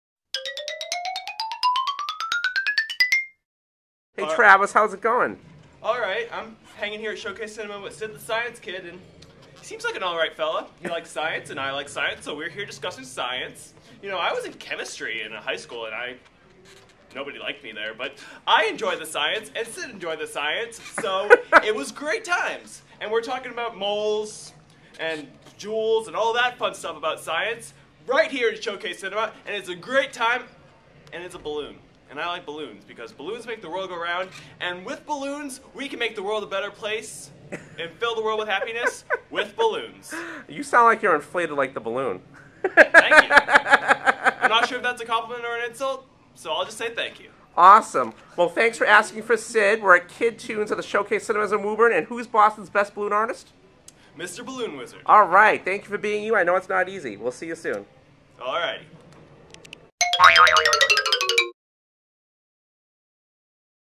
Sid the Science Kid Balloon
Boston's Best balloon Artist (Mr. Balloon Wizard) Mr. Balloon Wizard made "Sid The Science Kid" out of Balloons at The Showcase Cinemas in Woburn, Ma. for Kidtoons on Saturday May 12, 2012.  Mr. Balloon Wizard and Promo Magic will make your event one to remember! From Trade shows to Birthday parties!